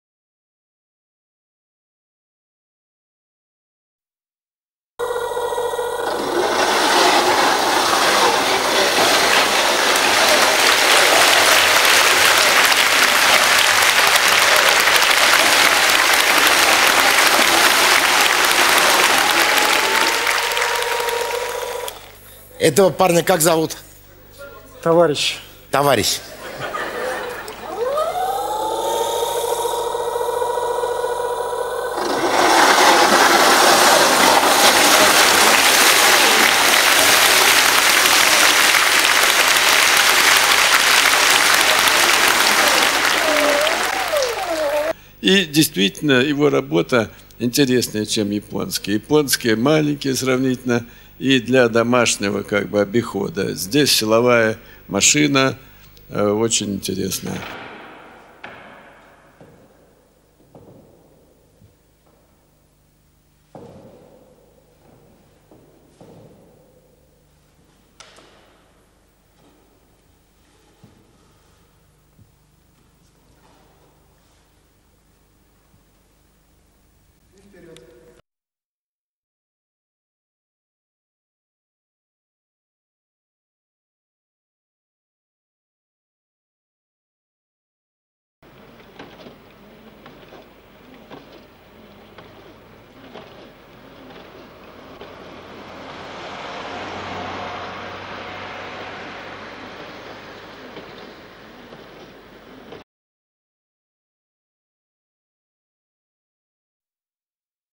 Russian Walking Robots
We are constructing the saddle robots with unique characteristics: Vmax-70km/h.; Mmax-2000kg. Japanese ASIMO is a kid.